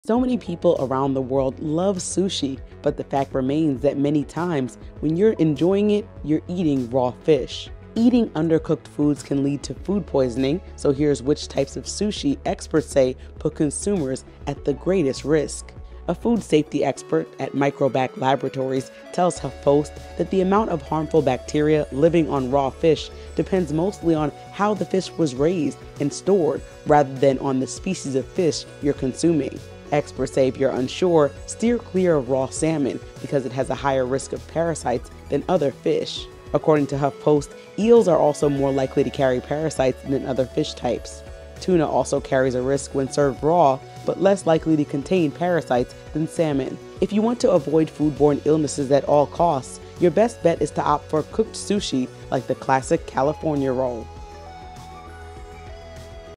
Skip This Type Of Sushi If You Want To Avoid Food Poisoning
So many people around the world love sushi, but the fact remains that many times when you’re enjoying it– you’re eating raw fish.